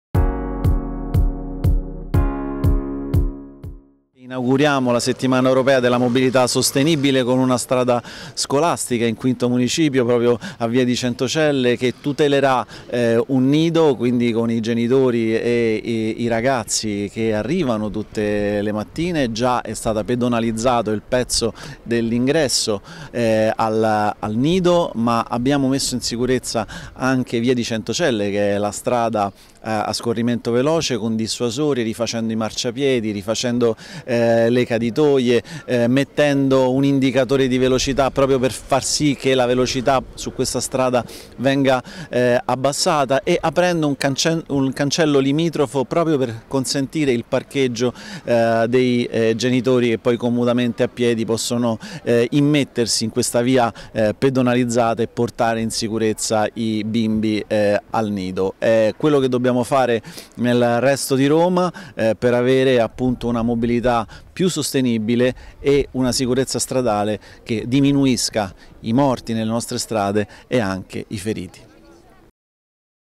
Strada Scolastica Centocelle
Dichiarazioni dell'Assessore alla mobilità di Roma Capitale Eugenio Patanè